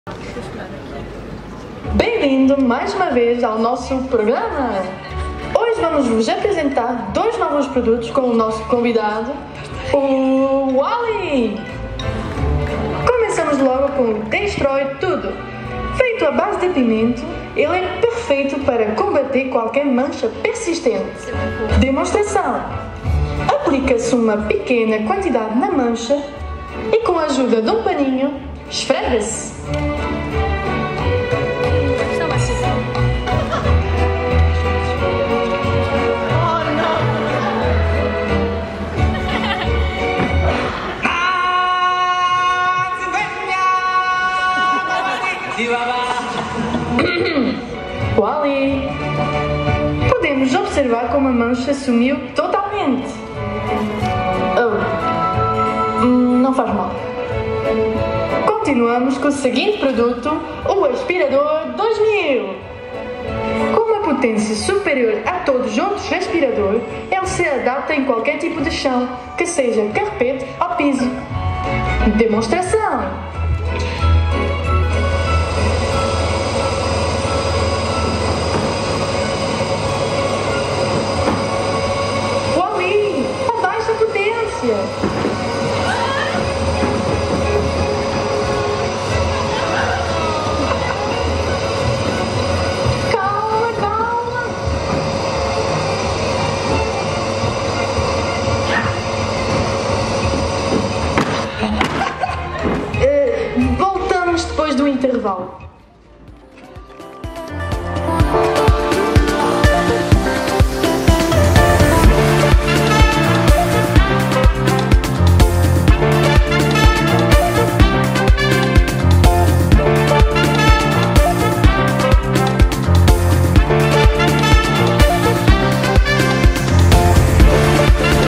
#07 Wall·e || Performers || Comic Con Portugal 2022
Concurso de cosplay Heróis do Cosplay Performers de la Comic Con Portugal 2022

Serie: Wall·e
Personaje: Wall·e